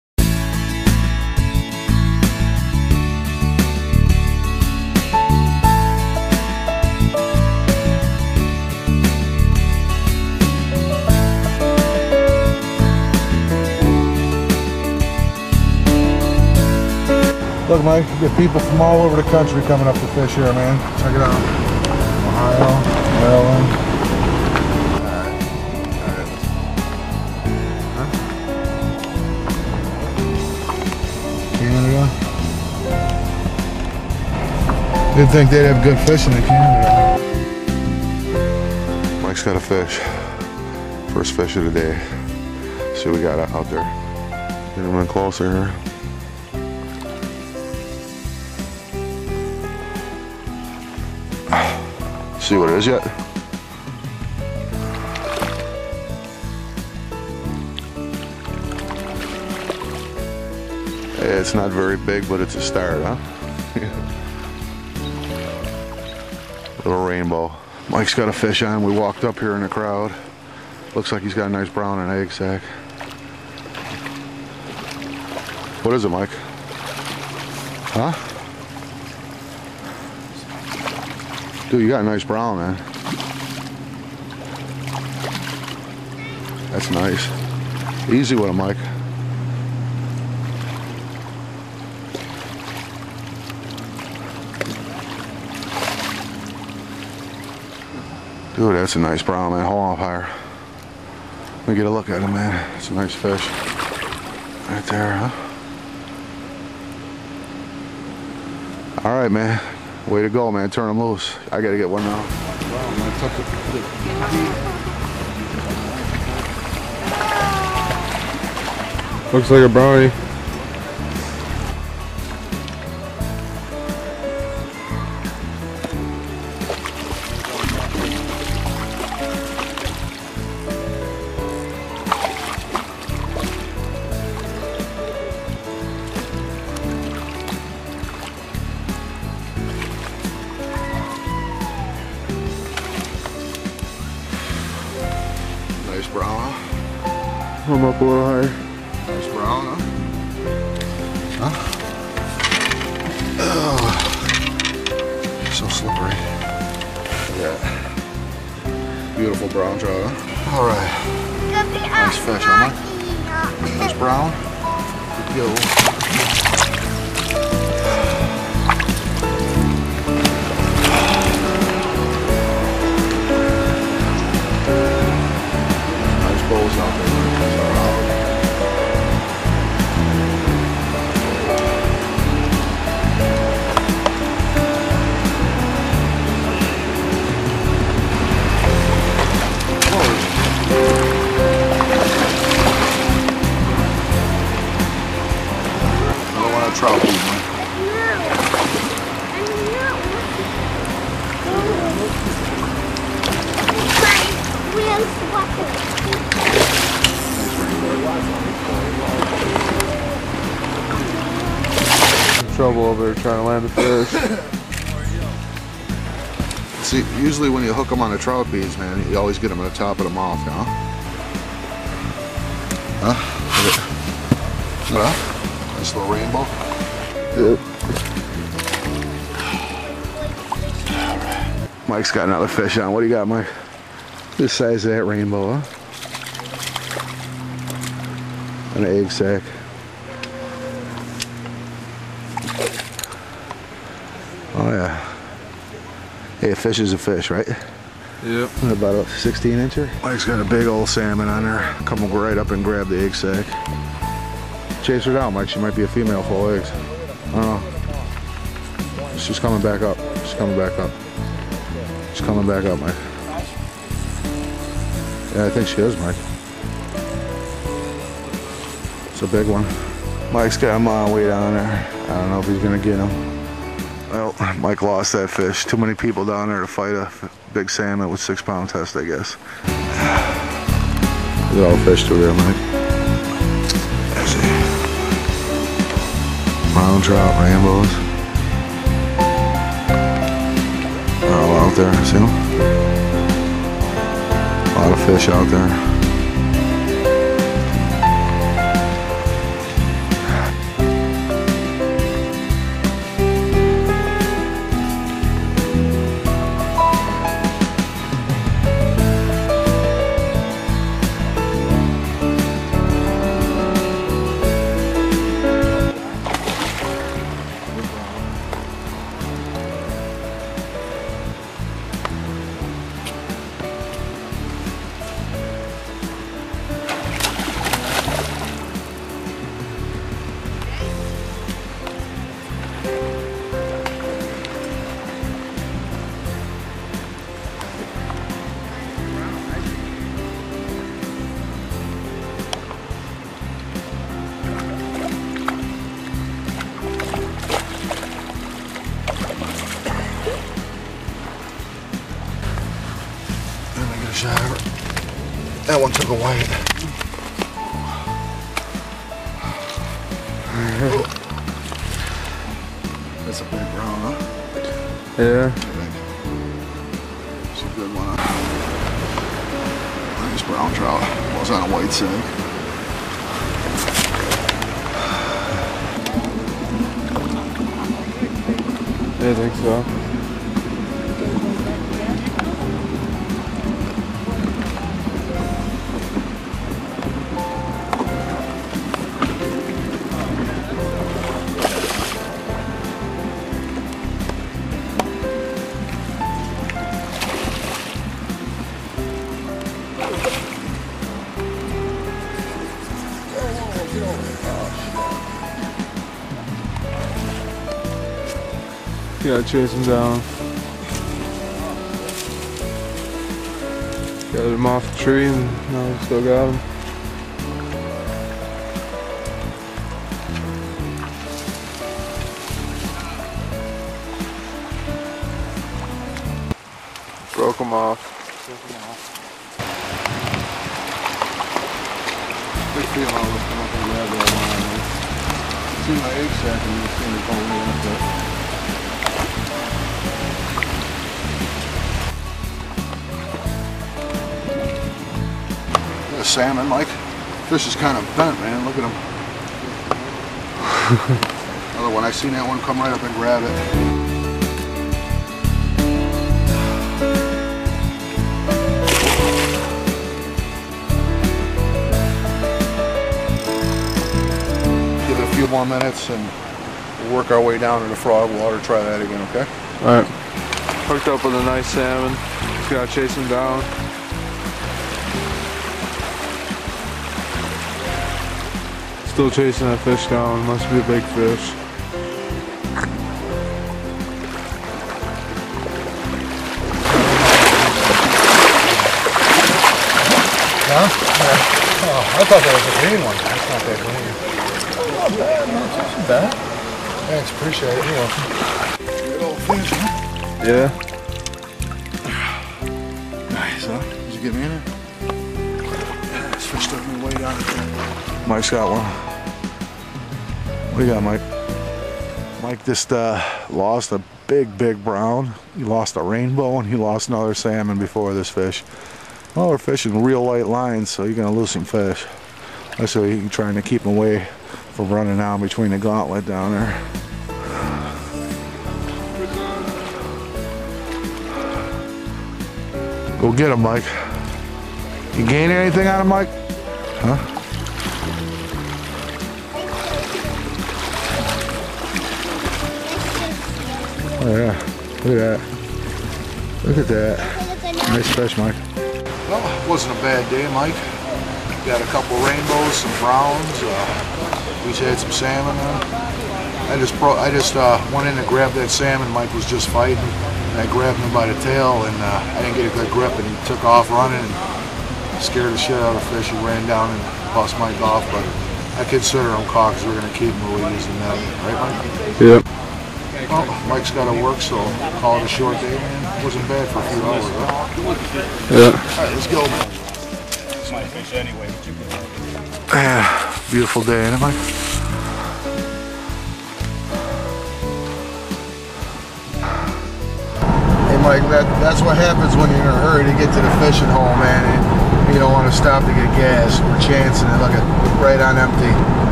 BROWN TROUT,BOWS & SALMON
trout fishing at burt / fishermans park -aka the zoo ,dirty burt,.
caught some nice browns ,  bows and salmon on light tackle & 6 lb test line -no 40 lb test  & Tuna rods here brother.

stop by the slippery sinker bait shop in olcott ,ny for all your stream fishing supplys.